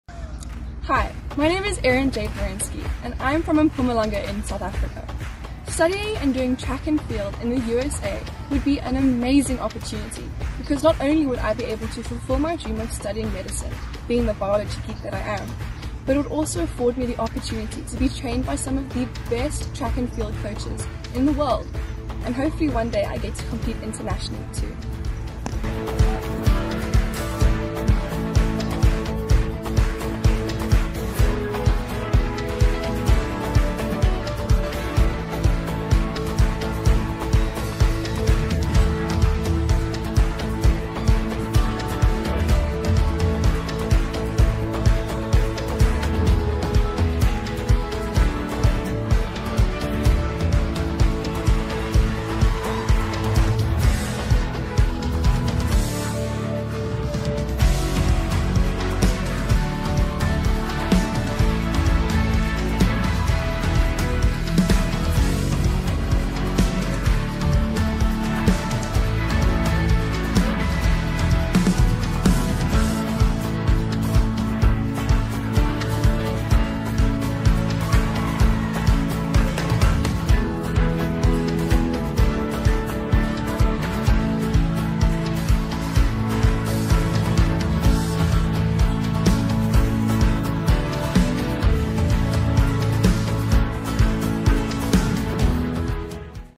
Erin Jade Merensky - USA University Women's Track and Field Recruit, Fall 2024
Height: 5'10”
Weight: 127 lbs
GPA: 2.86

Event Personal Best:
Long Jump - 5.80m
Triple Jump - 12.03m
100m Hurdles - 15.37s